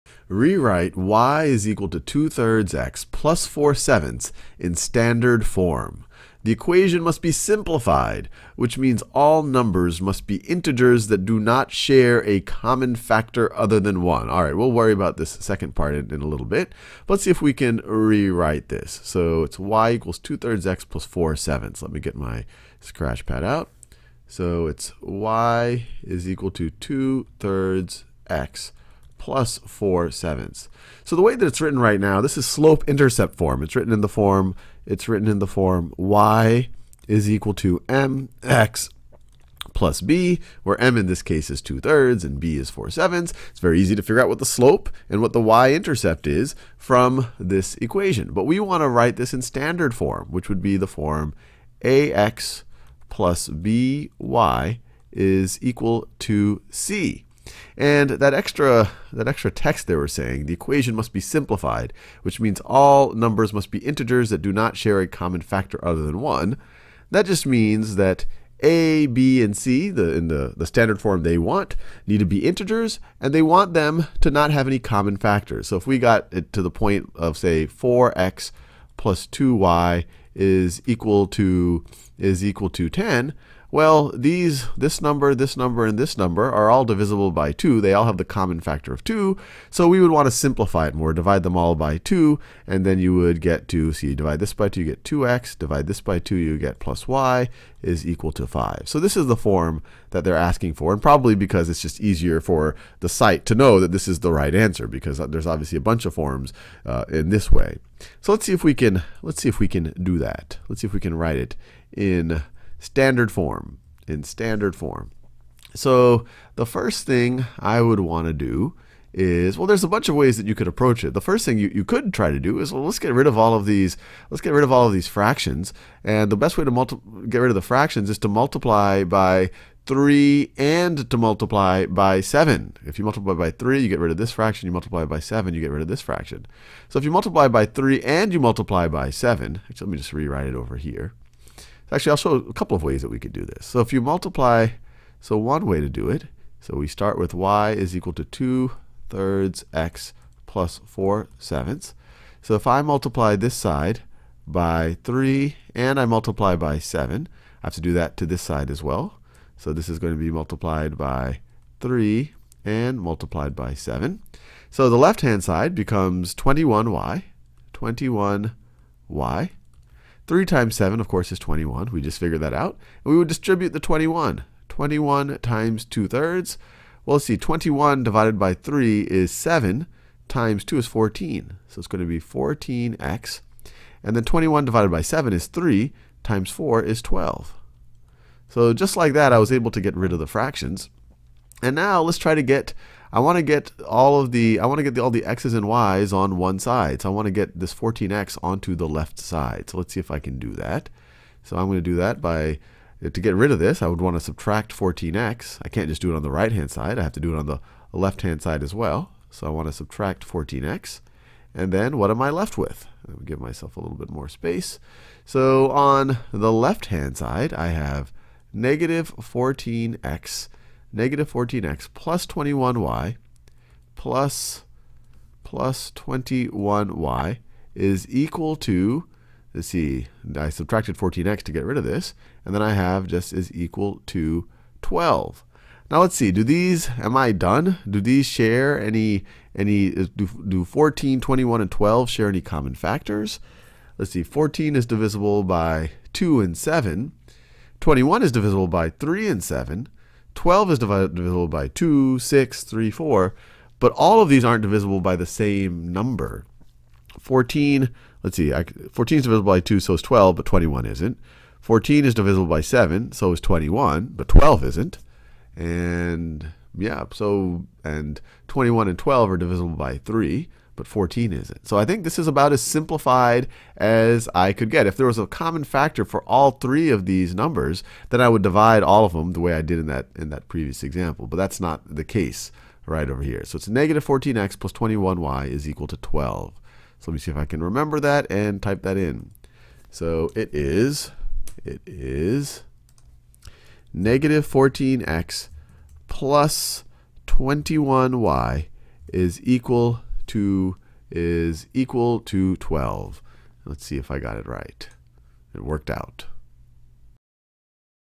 Converting from slope-intercept to standard form | Algebra I | Khan Academy
Courses on Khan Academy are always 100% free. Start practicing—and saving your progress—now

Converting from slope-intercept to standard form